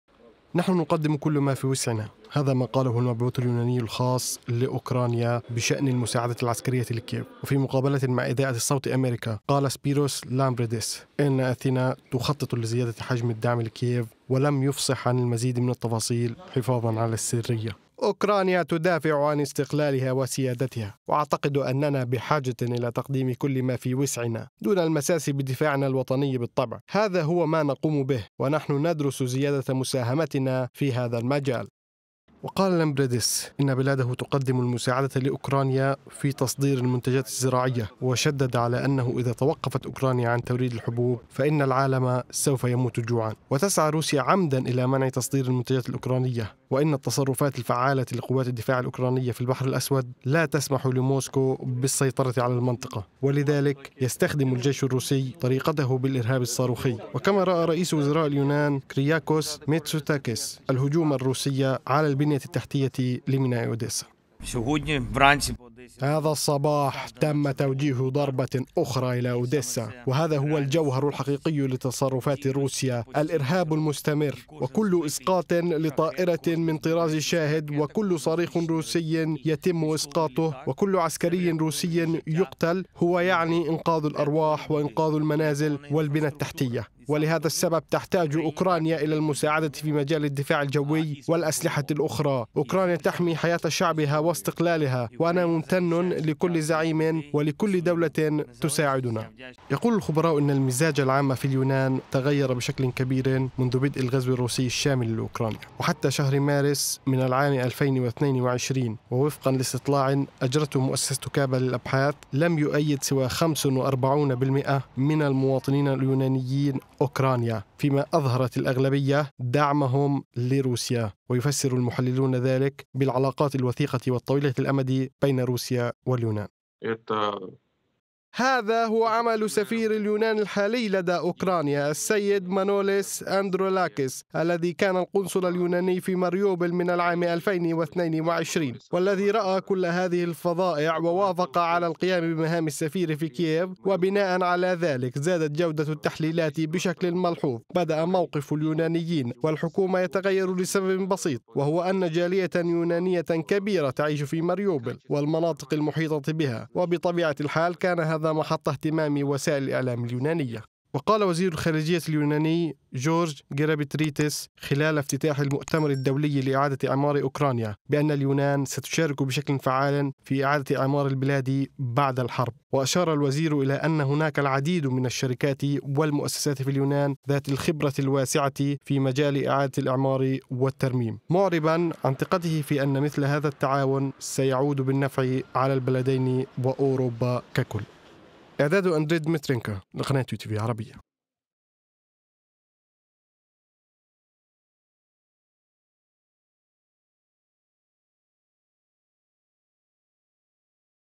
تخطط اليونان لزيادةمساعداتها لأوكرانيا
قالَ المبعوث اليوناني الخاص لأوكرانيا بأن بلاده تعمل على مساعدة كييف في تصدير المنتجاتِ الزراعية، وبأن أثينا تخططُ لزيادةِ حجم الدعم لأوكرانيا في عدد من المجالات المختلفة